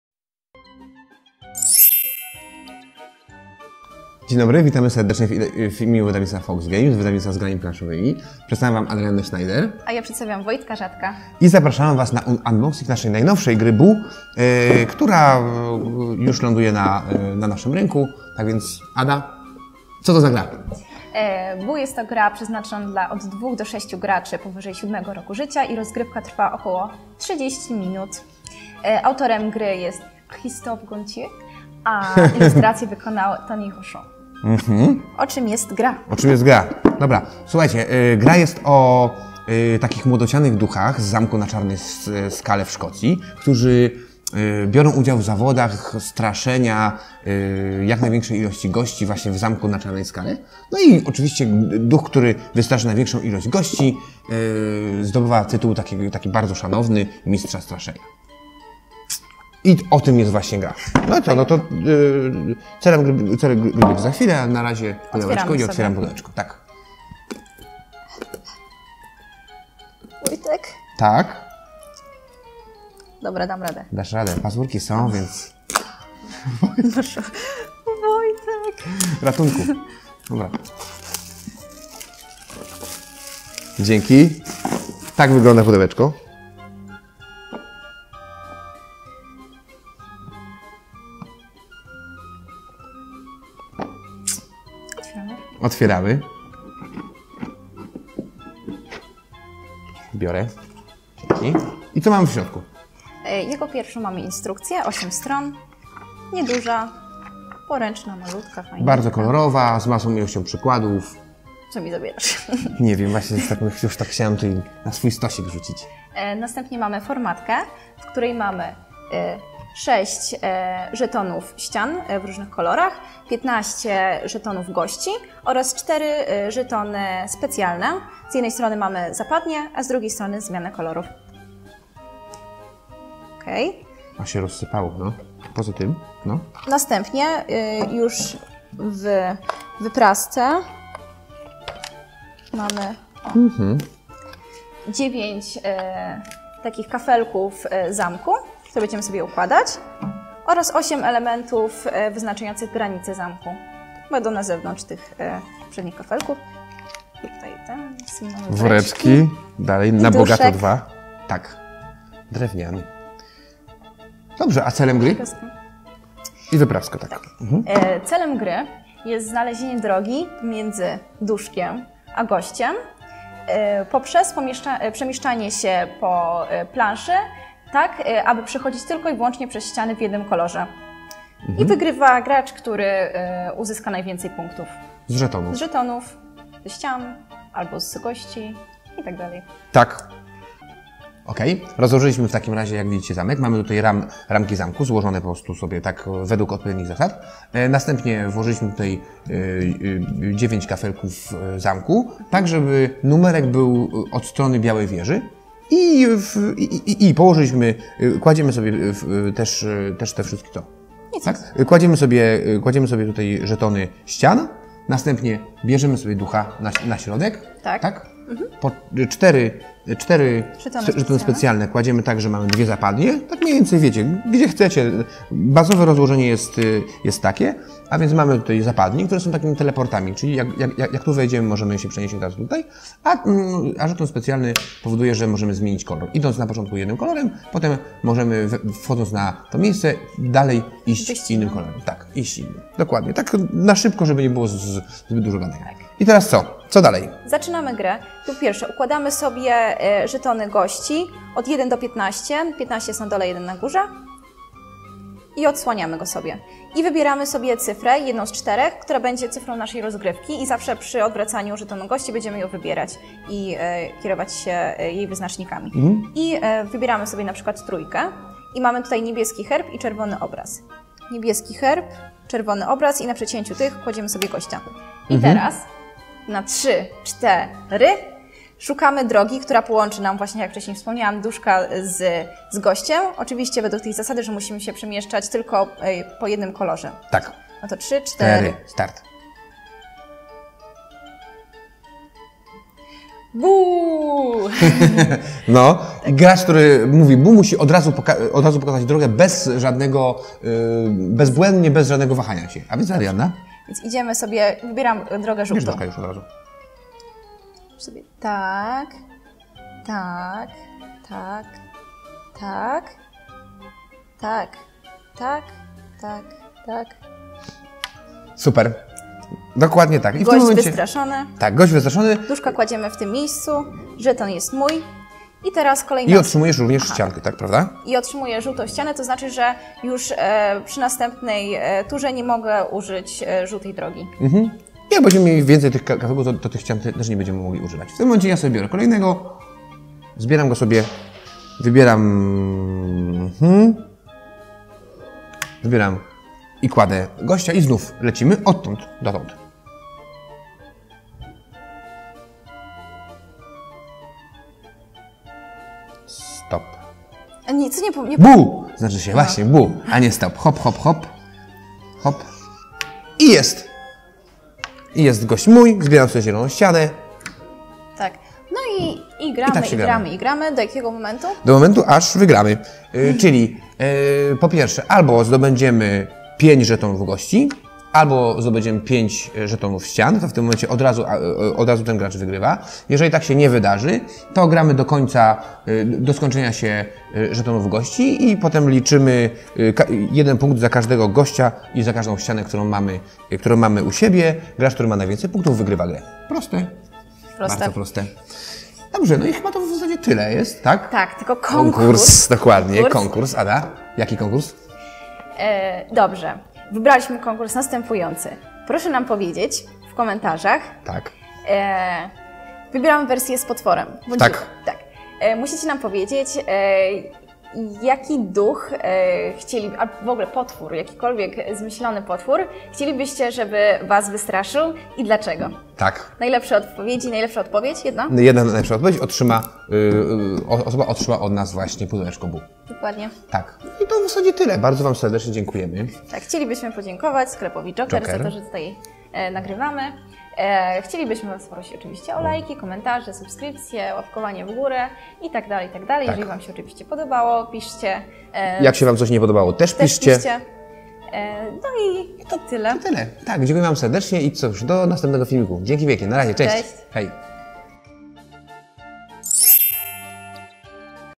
Buuu - Unboxing + konkurs + zasady gry
Unboxing i skrót zasad gry "Buuu!" wydawnictwa FoxGames oraz konkurs, w którym wygrać możecie grę "Buuu!".
Konkurs rozwiążemy 9 marca

Buuu to gra w której wszyscy grają jednocześnie. Celem jest jak najszybsze znalezienie drogi dla ducha, aby dotarł do gości "Zamku na Czarnej Skale" i porządnie ich nastraszył!

Gracze zagłębiają się w korytarze i tajemne przejścia, przechodzą przez ściany i zmieniają kolory! Ten kto okaże się najszybszy zdobędzie tytuł "Mistrza Straszenia"!

Zapraszamy do zapoznania się z grą i obejrzenia filmiku!

Zapraszamy również do konkursu :)
1. Pytanie /konkurs znajdziecie w filmiku :) - bądźcie uważni podczas oglądania :)
2. Nagrodą jest gra Buuu
3. Odpowiedzi wpisujcie w komentarzach pod filmem
4. Wygrywa jedna najciekawsza odpowiedź, czyli jedna osoba
5. konkurs zaczynamy już teraz, a kończymy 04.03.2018 roku o godzinie 23:59
6. wyniki ogłosimy 9 marca 2018r.

Gotowi?
ZACZYNAMY!!!